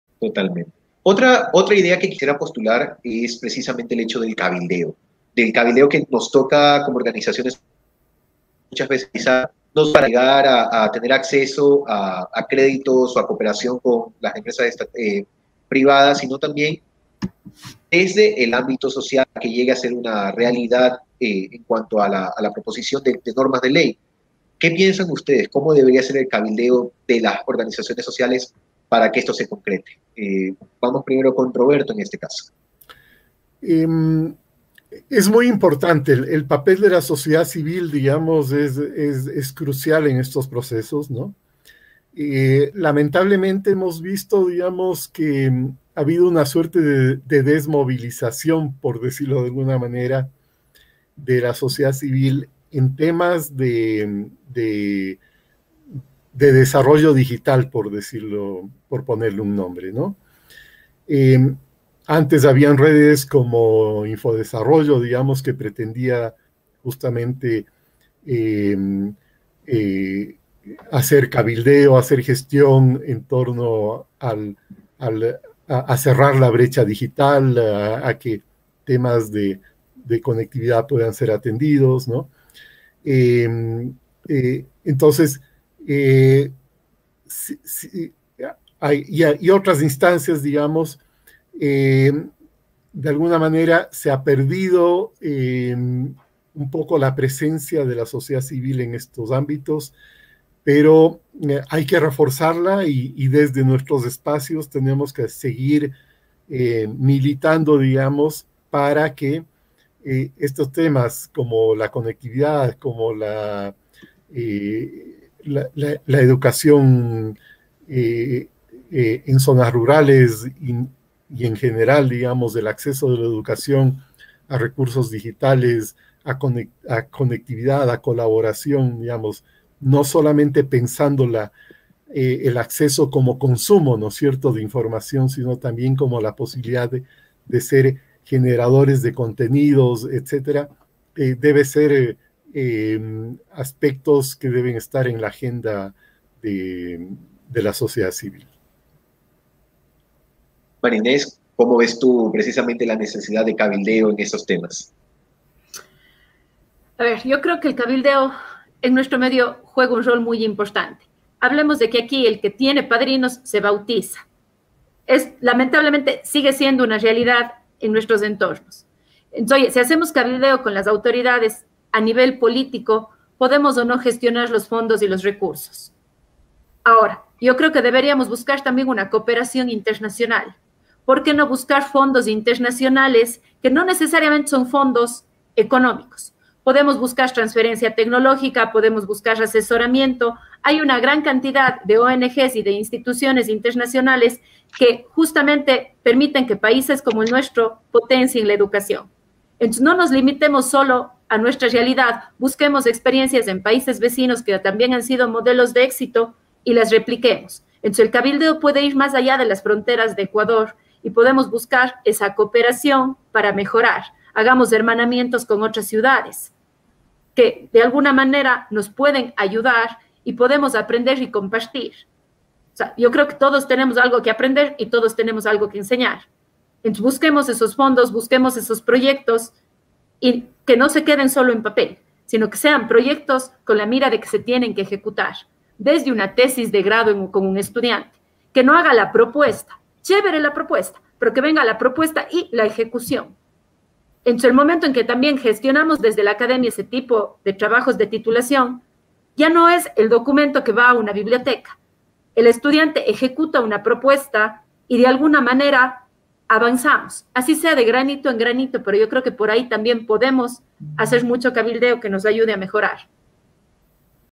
Cómo debería ser el cabildeo de las organizaciones sociales para concretar políticas públicas
Extracto de la 11ª sesión de "Educación libre", "Desafíos para implementar una política de educación abierta".

Con la moderación de Edgar Rosero, María Inés Acosta y Roberto Roggiero reflexionan acerca de la forma en el que las organizaciones sociales deberían realizar el cabildeo para conseguir que se concreten políticas públicas de educación libre en la ruralidad.